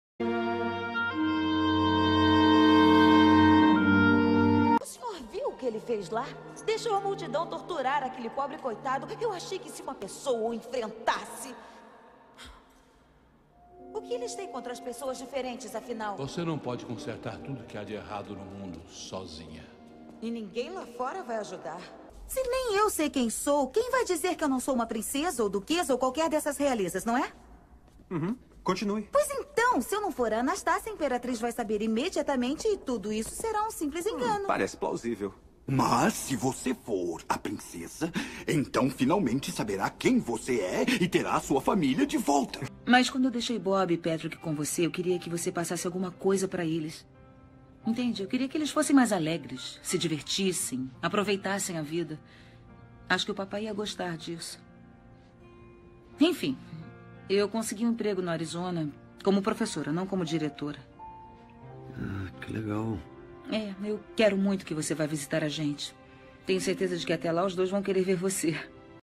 MÔNICA ROSSI
A homenageada de hoje tem uma ótima voz para mulheres de personalidade forte. Eis aqui uma breve amostra do trabalho da grande Mônica Rossi.